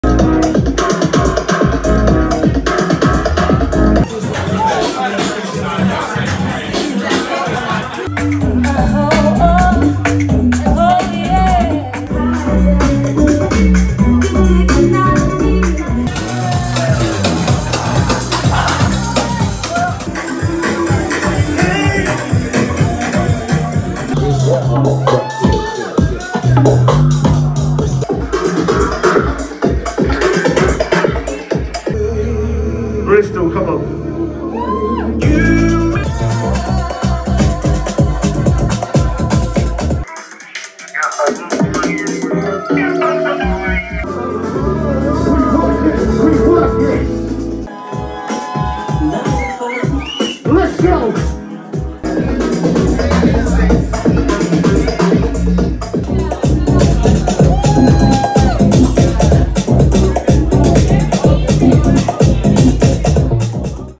A Movement at CLOAK BRISTOL MARCH 2024 RECAP
Bristol’s A Movement returns with another monster event at CLOAK. Full crew Sean McCabe, Boogie Cafe,  Sentinel793, Haze City and special guest EVM128 tearing it up with the bruk riddim.

Pure pressure on the dance floor. If you ever up north and want to get down to broken beat, house, jazz, future soul, bruk, look no further. A Movement gots you.

- iMantús/DJ Psykhomantus

🎥 iMantús